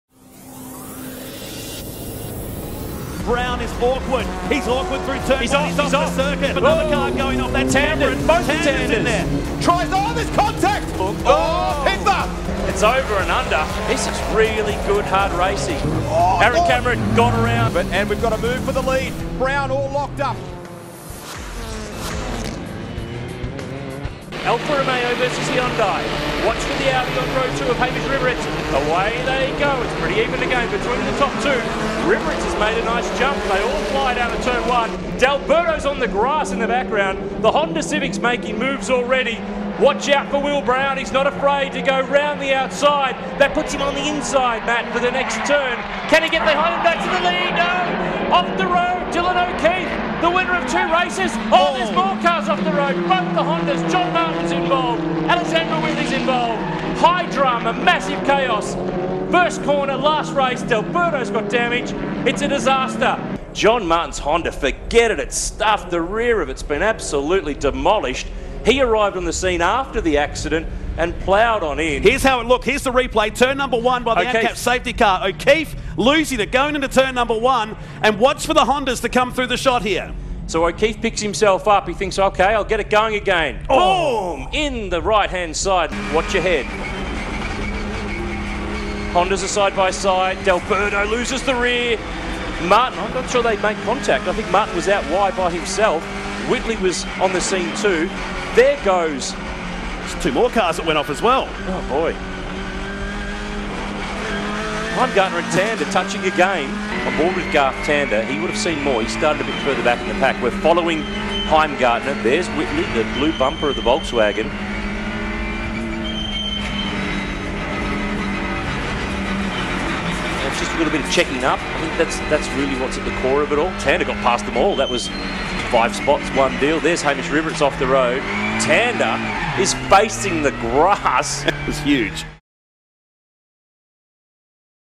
2019 Top 10: High Drama. Massive Chaos
It was only the sixth race of the season, and there was already a highlight reel full of major accidents. This one was the biggest of 2019.